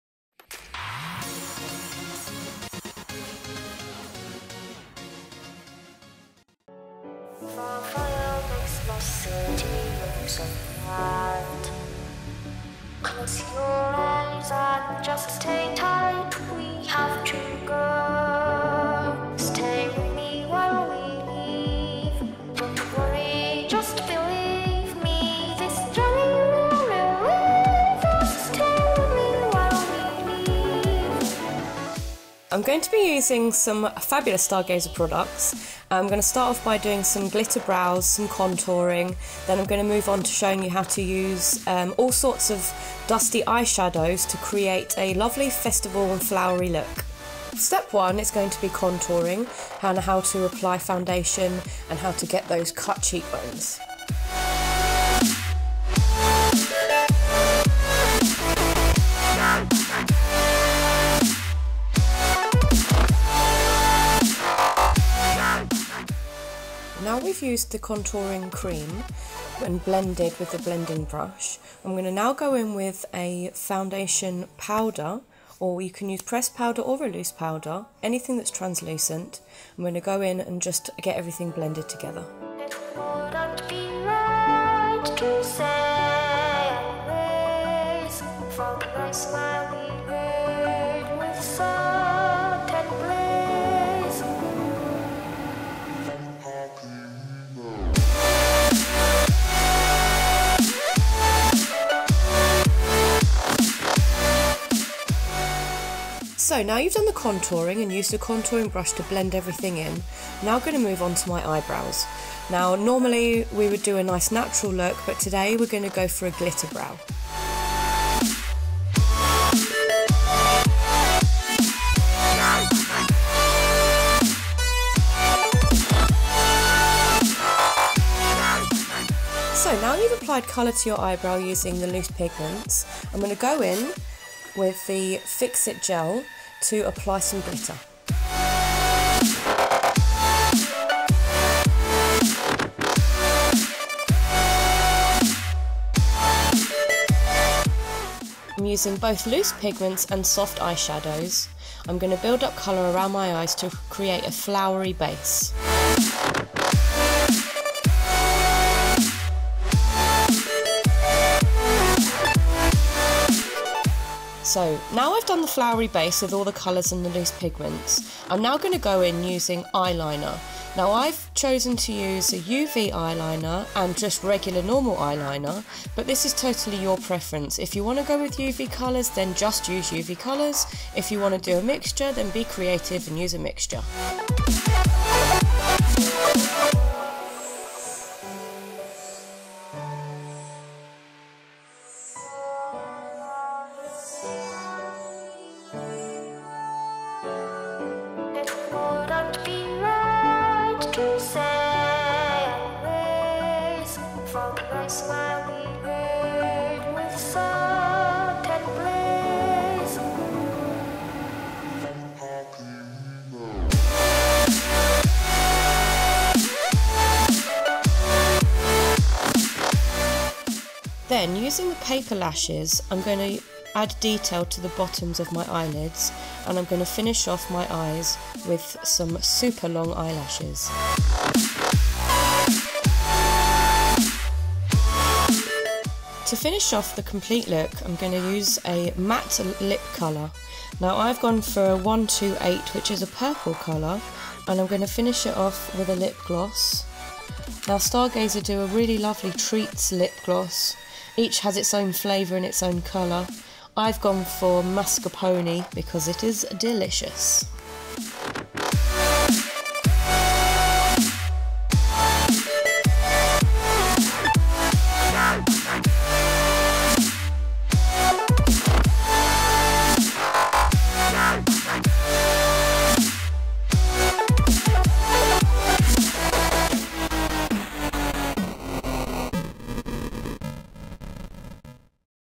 Stargazer Presents... Spring Flower Festival Face Art
Festival season is fast approaching and Stargazer products are perfect to create those bright, colourful festival looks.

To welcome in the festival calendar we teamed up with the wonderful Re_Fab to bring you this tutorial so you too can create your very own festival flower face art.

Glitter Brow – Re_Fab uses loose pigments on the brow to achieve the colour. These are also known as Eye Dust. She then used Fix Gel and applied the glitter.

Eye Dust and Soft Eye Shadow were used on the eyes to create the flowers - with UV Liquid Eye Liner to define the flower detail.

Re_Fab then used a Matte Lipstick – 128 (purple) finished off with a Treats Lip Gloss.